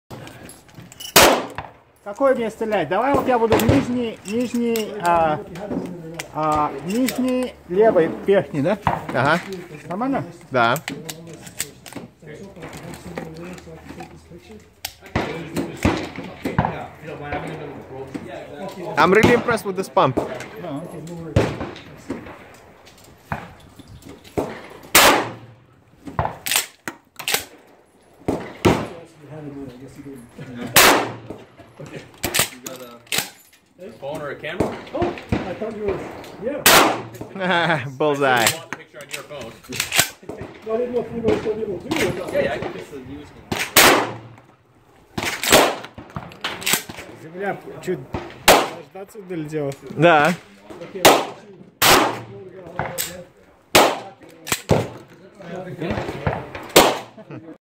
Canuck Regulator Part 13
Canuck Regulator Pump Shotgun
These new shotguns from Canuck are new to the Canadian market. This particular model sports a 14" barrel. It feels very handy and points extremely naturally. With a capacity of 4+1 2-3/4 12 Gauge shotgun shells make this one great range toy or bush gun. This combo deal comes with both a fixed stock and a bird head grip!
Specifications:
• Gauge: 12
• Chamber: 2-3/4" or 3"
• Capacity: 4+1
• Sights: Fixed Front
• Barrel Length: 14"
• Choke: F,M,IC
• Stock: Synthetic Bird Head Style Grip and Fixed Stock
• Overall Length: 26.5"
• Weight: 4.6 lbs.